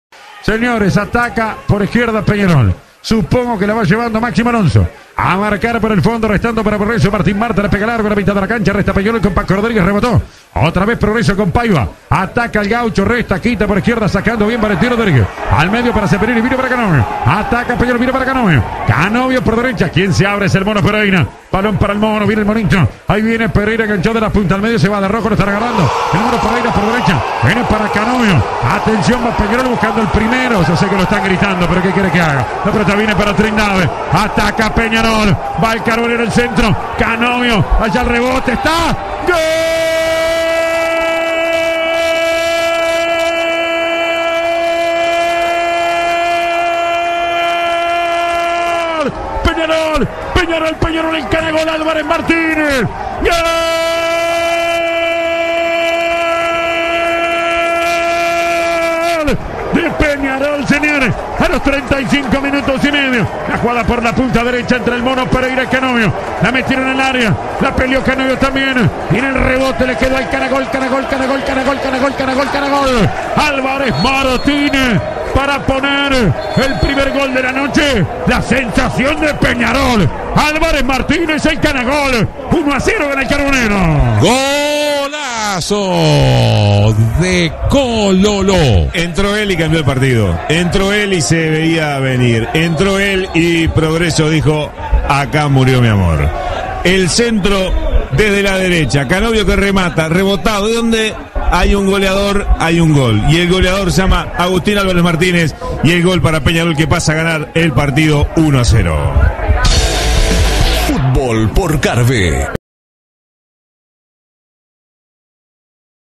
Peñarol 1 Progreso 0 - Apertura 2021 (por Gabriel Regueira)
El gol de Agustín Álvarez Martínez, por Gabriel Regueira y Martín Díaz para CX 16 Radio Carve 850 AM.